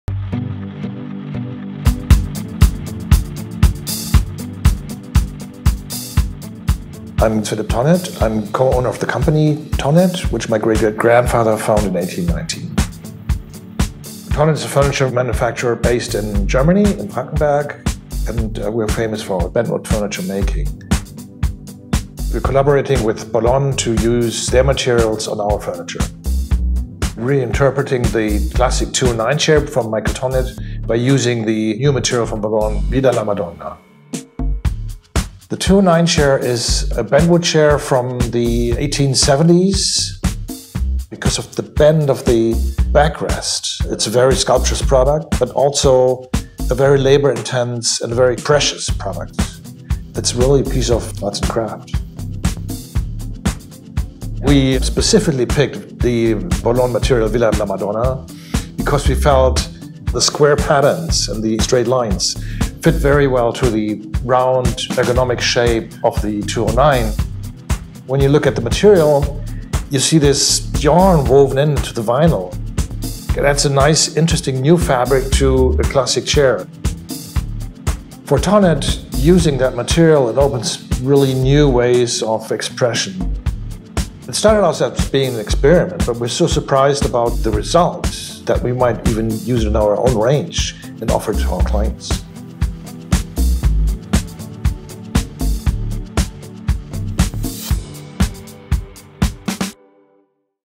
Innovators at Heart #8: Bolon x Thonet
Innovators at Heart is a conceptual series exploring the world of Bolon. Made in collaboration with Dezeen.

This is the sixth part exploring the collaboration together with renowned German design company Thonet.